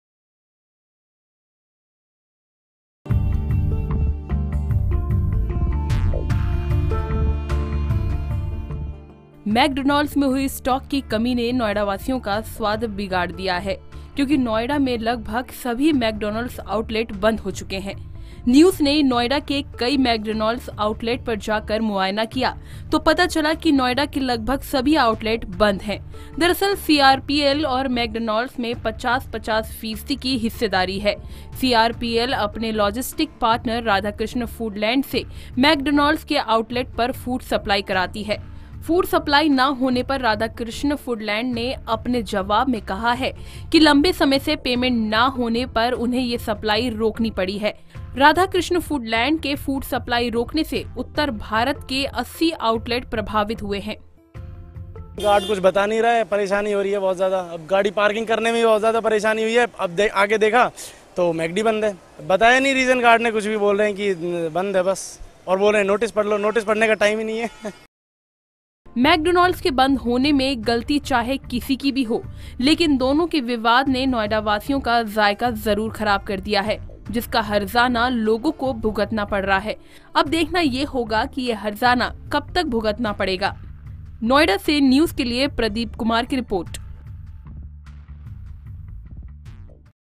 Noida में बंद हुए सभी McDonald's | Big News Of Noida | NYOOOZ UP
McDonald's में हुई स्टॉक की कमी ने नोएडावासियों का स्वाद बिगाड़ गया है क्योंकि नोएडा में लगभग सभी मेकडॉनल्ड्स ऑउटलेट बंद हो चुके है। न्यूज ने नोएडा के कई मेकडॉनल्ड्स ऑउटलेट पर जाकर मुआयना किया तो पता चला कि नोएडा के लगभग सभी ऑउटलेट बंद है |